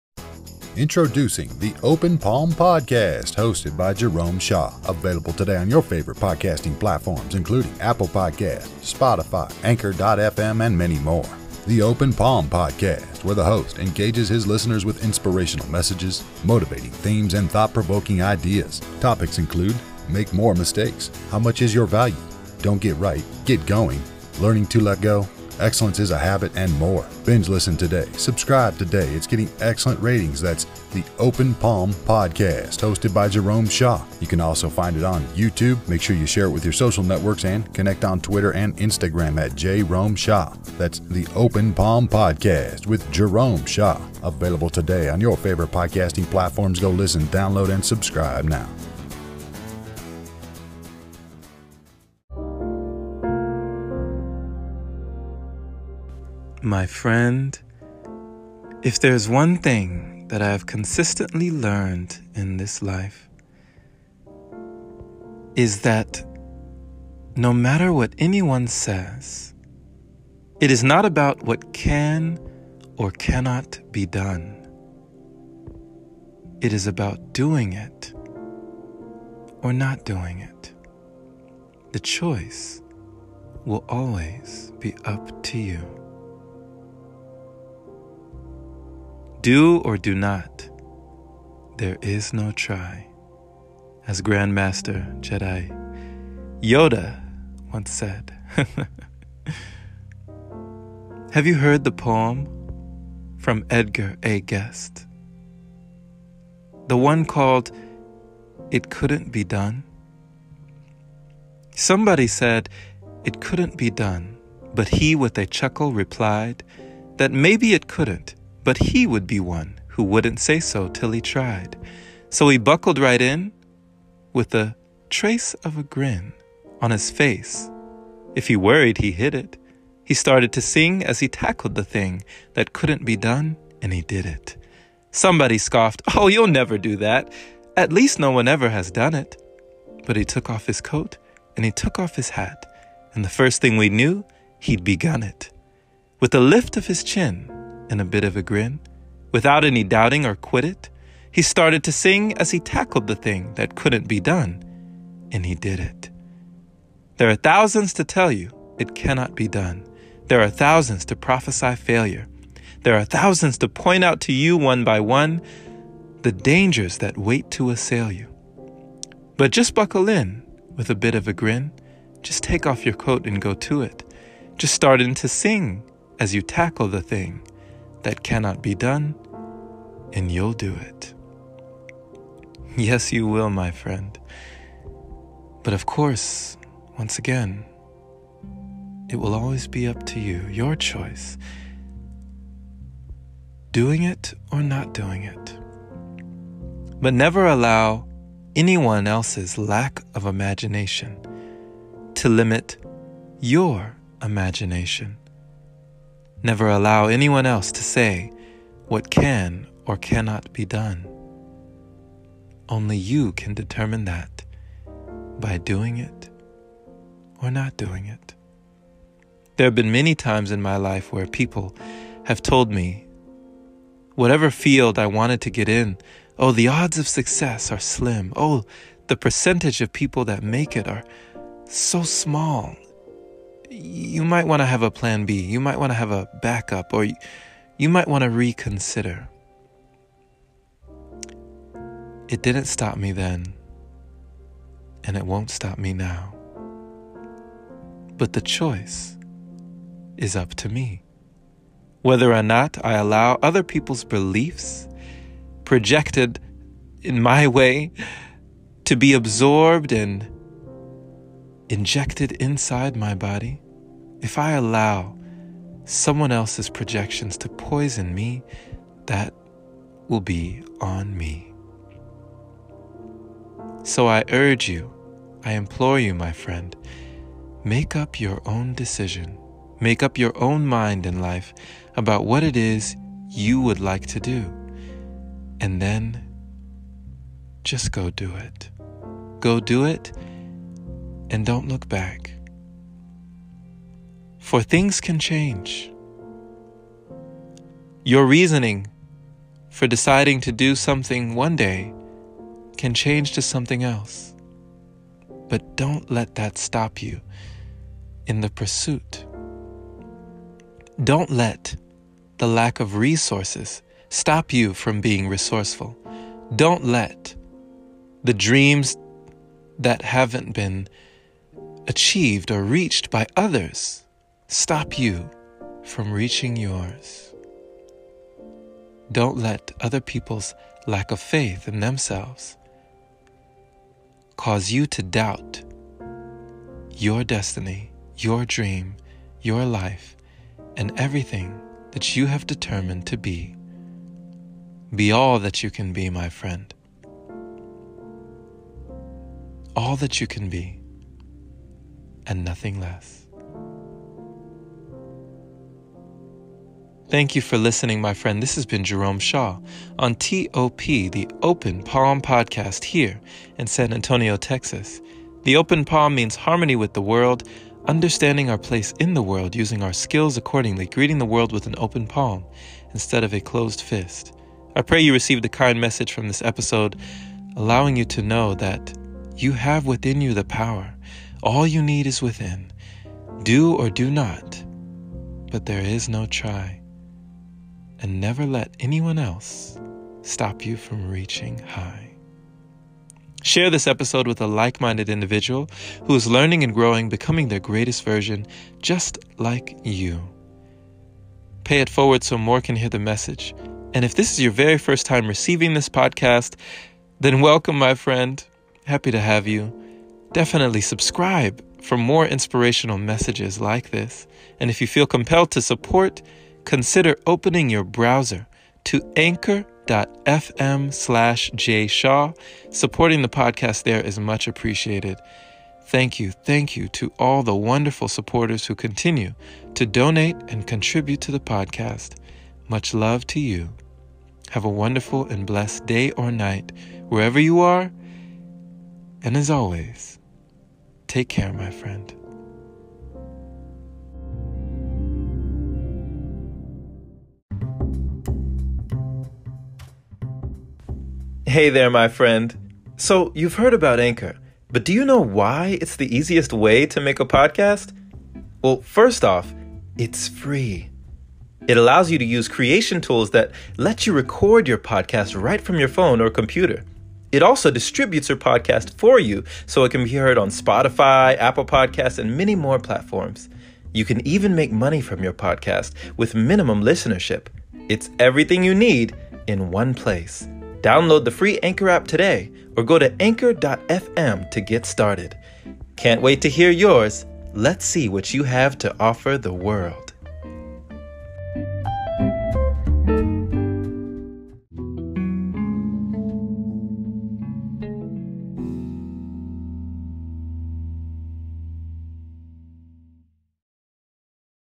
Doing or Not Doing
The Open Palm // Regardless of what anyone else says. The choice will be up to you to chase your dreams and make them a reality or not. Never let someone else’s lack of imagination limit your imagination. Once you make a decision, go and do it. Don’t wait for a good time. There is never a perfect moment. Treat now, today, as all you have. If you fall that’s okay. But give it your all. Life will be what you make it. Write your own story and keep the pen close. Grab the new book on AMAZON! 📚